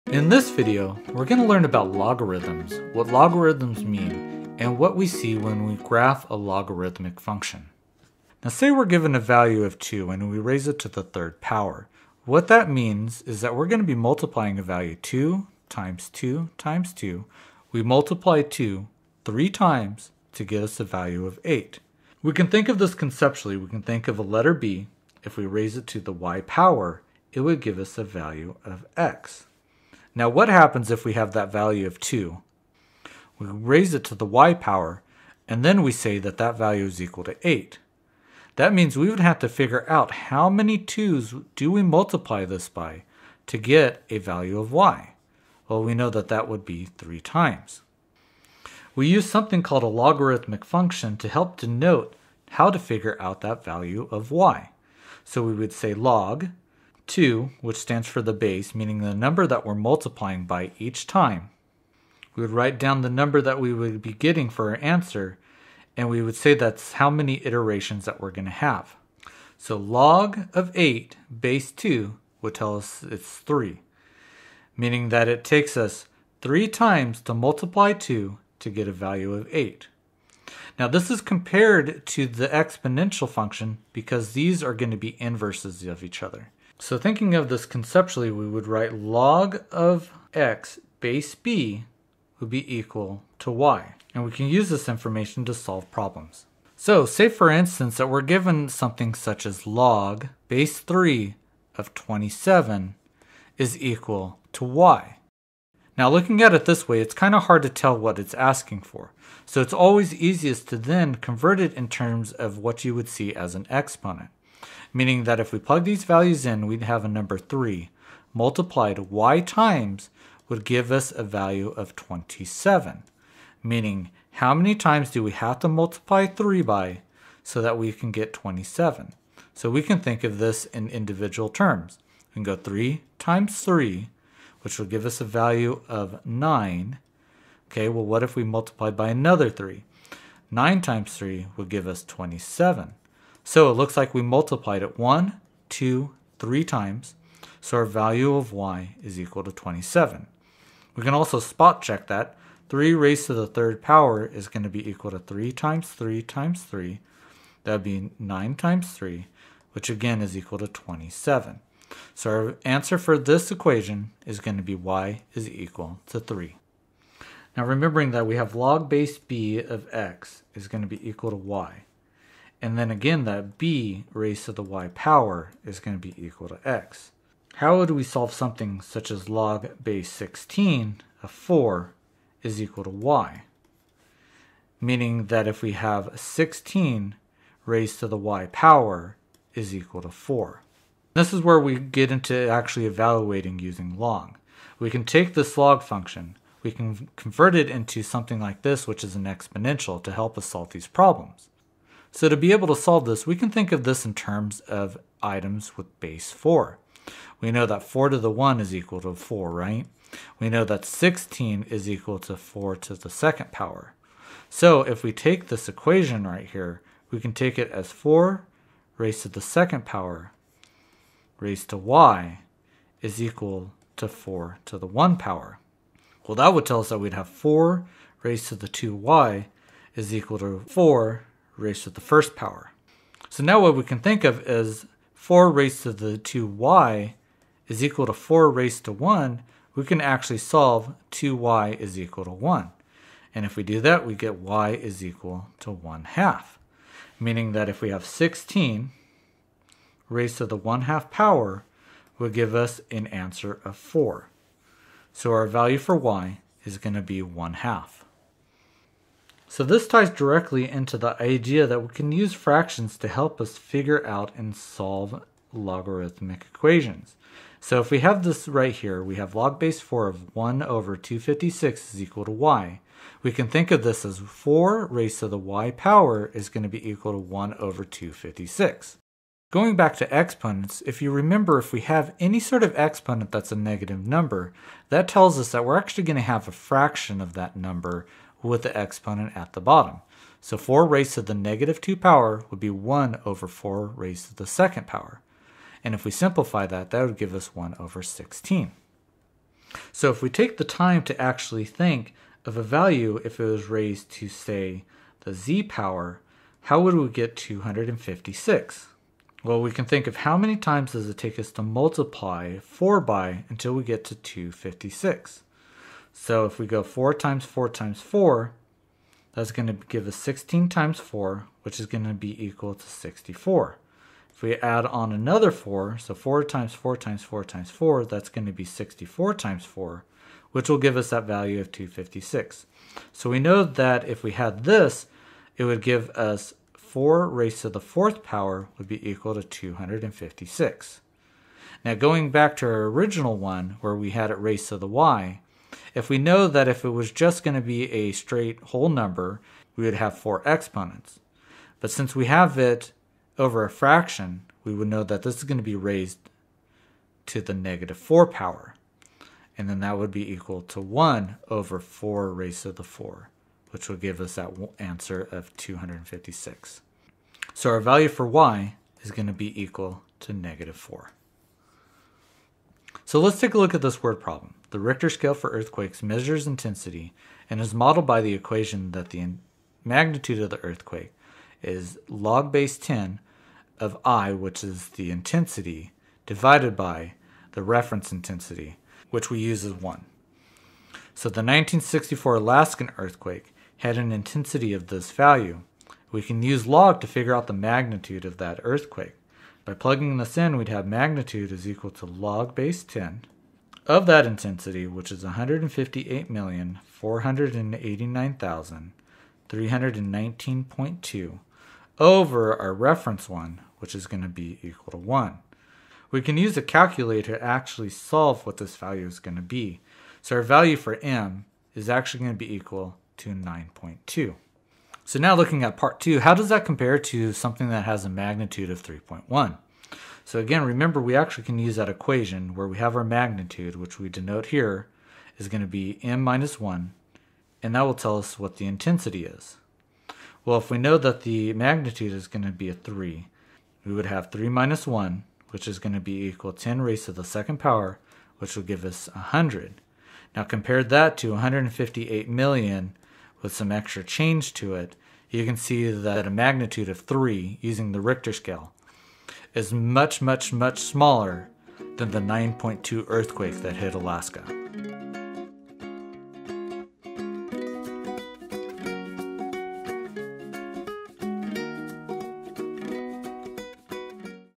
Solving Equations Using Logarithms
In this video, we break down logarithmic functions from the ground up. We’ll solve log₂ 8 = x, log₁₆ 4 = y, and apply logarithms to a real-world Richter scale earthquake problem involving the 1964 Alaska earthquake. You’ll learn how logs work, how they relate to exponents, and why they’re essential for math, science, and technology.

Video Chapters:
00:00 Introduction
00:08 Concept Overview
01:37 Practice Problem 1
02:47 Evaluating Using Logs
04:23 Fractions Using Logs
06:12 Word Problem - Part 1
07:05 Word Problem - Part 2